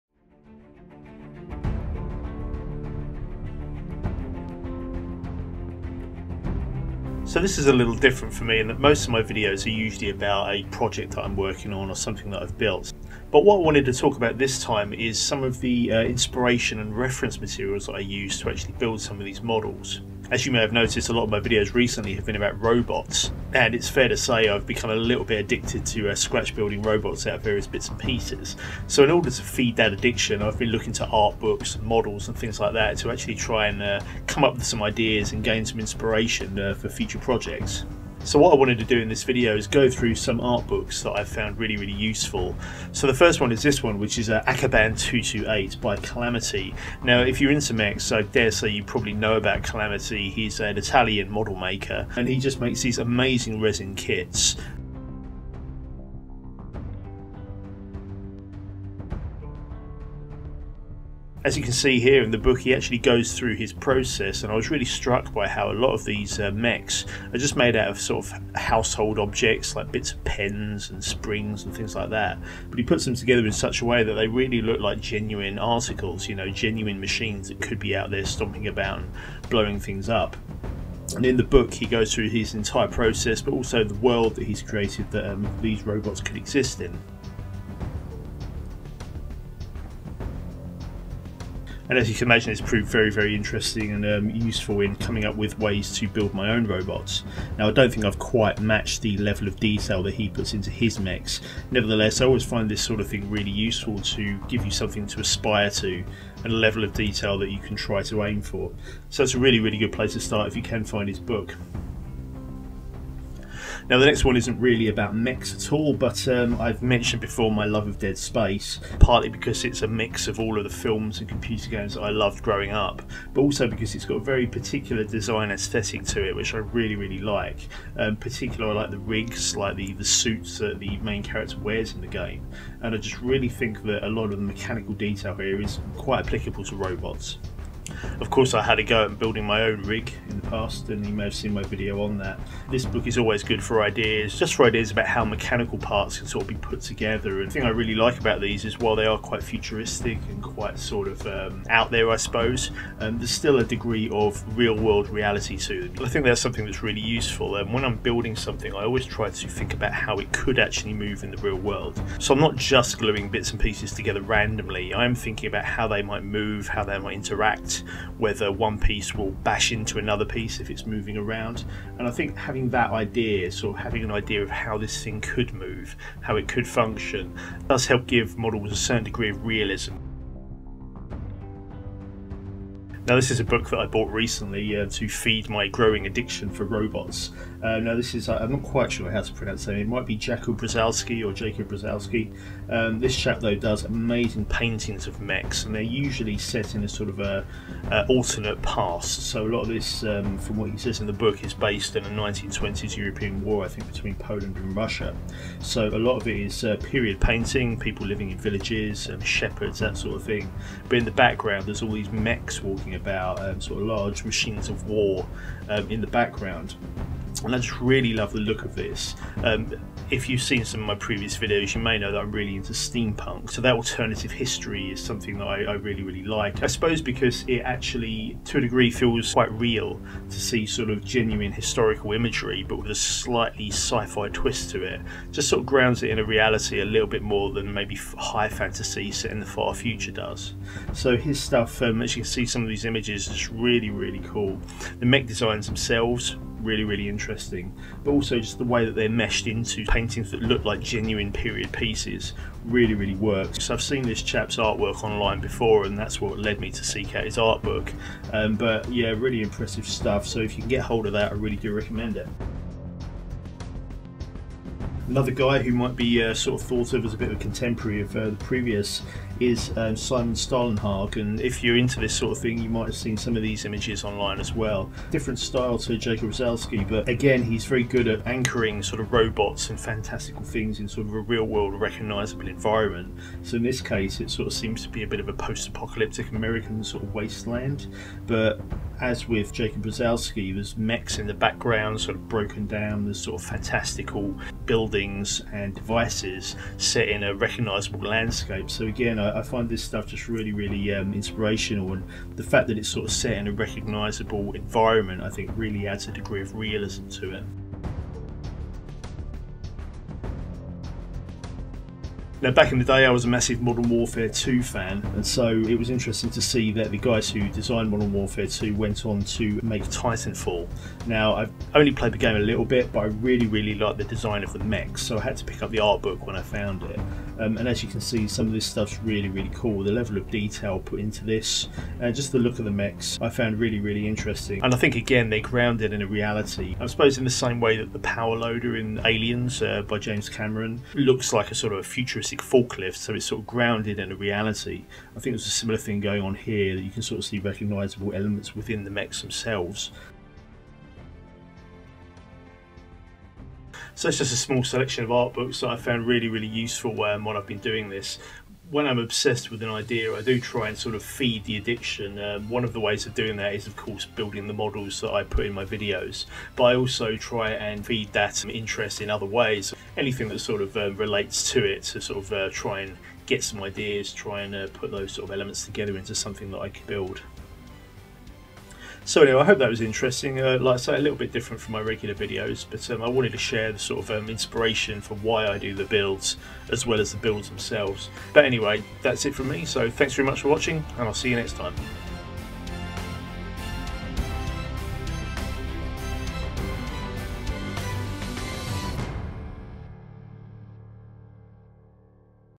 Art Book Inspiration for Mech Scratchbuilds
A quick look at some art books that have inspired me to build some recent mech models.